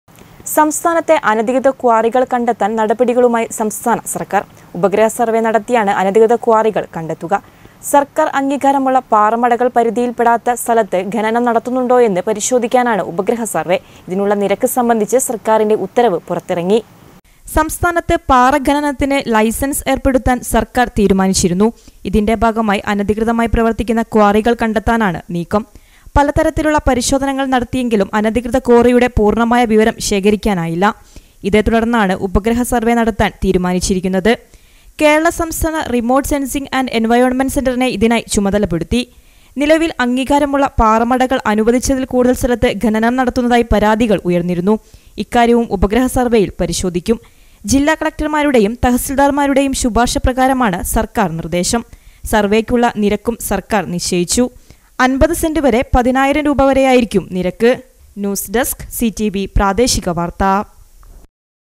സംസ്ഥാനത്തെ അനധികൃത ക്വാറികള്‍ കണ്ടെത്താന്‍ നടപടികളുമായി സംസ്ഥാന സർക്കാർ
സംസ്ഥാനത്തെ അനധികൃത ക്വാറികള്‍ കണ്ടെത്താന്‍ നടപടികളുമായി സംസ്ഥാന സർക്കാർ. ഉപഗ്രഹ സര്‍വേ നടത്തിയാണ് അനധികൃത ക്വാറികൾ കണ്ടത്തുക. സര്‍ക്കാര്‍ അംഗീകാരമുള്ള പാറമടകള്‍ പരിധിയില്‍പ്പെടാത്ത സ്ഥലത്ത് ഖനനം നടത്തുന്നുണ്ടോ എന്ന് പരിശോധിക്കാനാണ് ഉപഗ്രഹ സര്‍വേ. ഇതിനുള്ള നിരക്ക് സംബന്ധിച്ച് സര്‍ക്കാരിന്റെ ഉത്തരവ് പുറത്തിറങ്ങി.